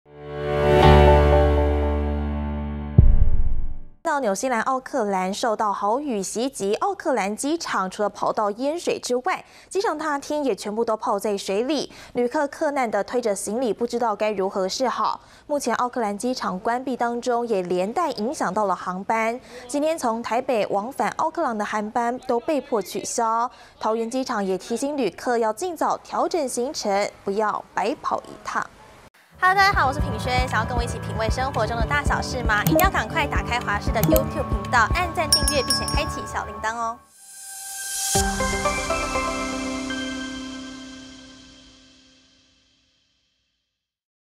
豪雨水淹奧克蘭機場 紐航往返台灣班機取消｜華視新聞 20230128
紐西蘭奧克蘭受到豪雨襲擊，奧克蘭機場除了跑道淹水。